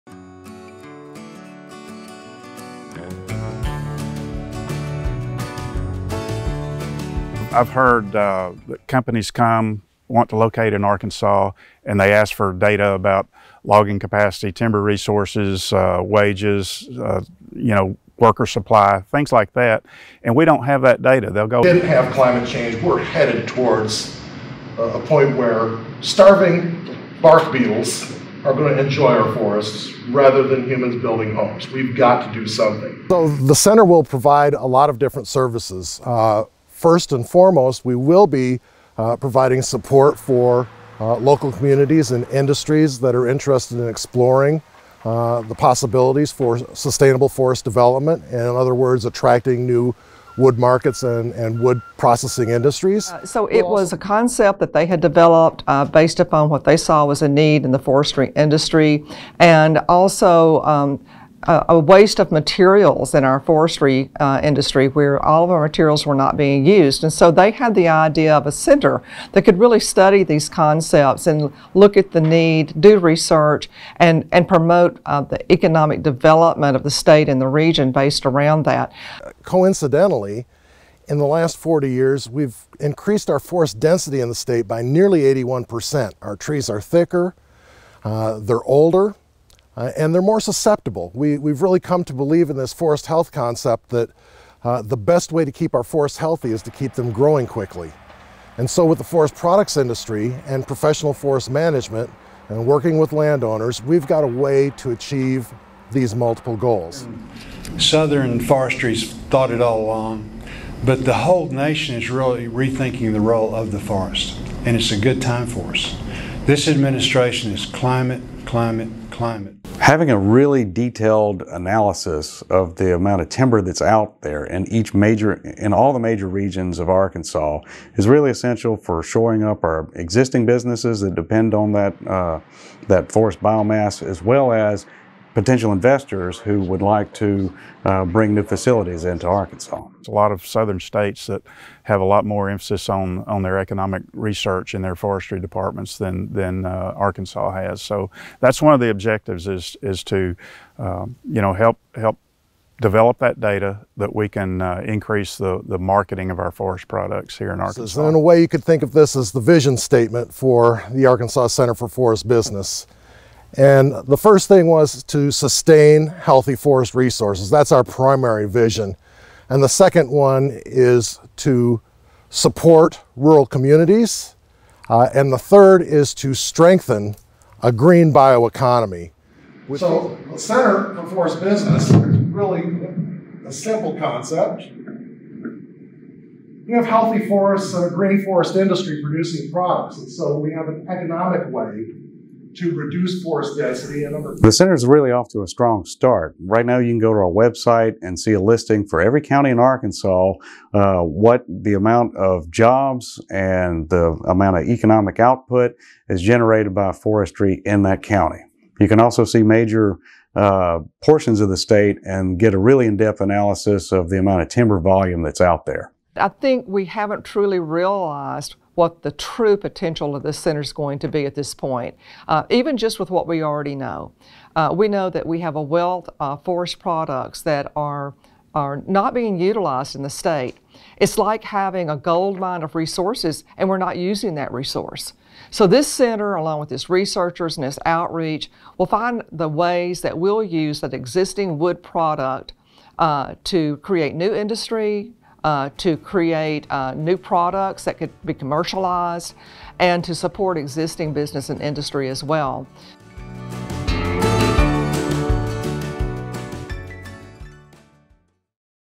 Arkansas Center for Forest Business Presentation
The newly launched Arkansas Center for Forest Business is seeking permanent funding after receiving one year of support from Governor Hutchinson.
Dr. Matthew Pelkki explains to community leaders how the overabundance of forests and timber now presents us with an economic opportunity. Pelkki explained why now the time is to move forward with the marketing project. He says census figures indicate declining populations and jobs in the region. Pelkki sees the ACFB as an opportunity to attract new wood products industry.
Not only would the new industry be producing wood-based products, but it would help manage what is increasingly becoming an insect haven and fire hazard. Pelkki can’t emphasize enough that now is the time to harvest more timber and turn it into marketing material. (TVT 4:19)